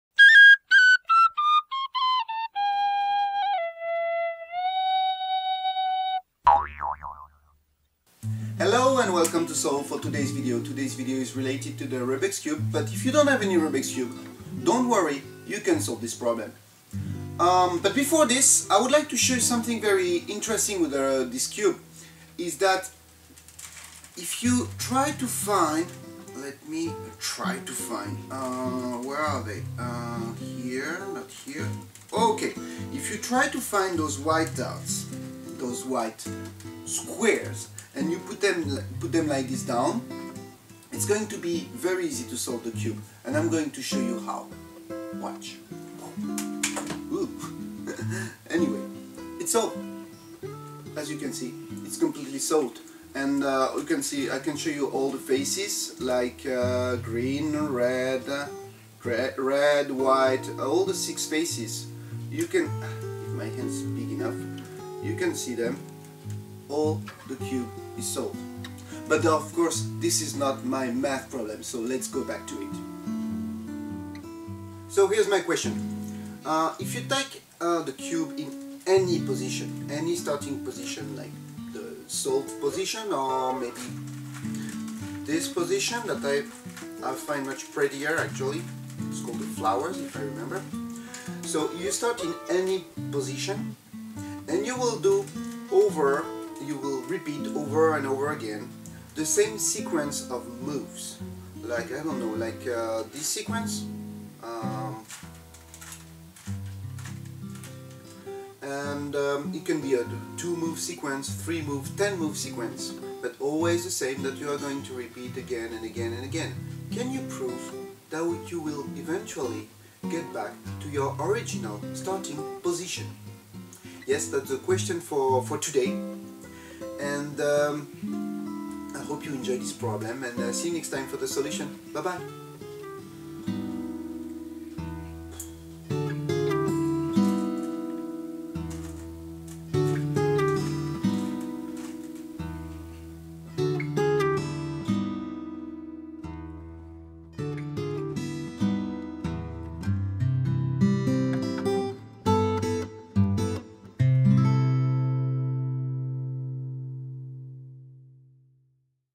Rubik's Cube Magic and Math Problem
Here's a new problem. I got it... from a dream! I dreamt of this problem, isn't it amazing? Rather scary, actually.
Enjoy it and don't get nightmares out of it!
with permission.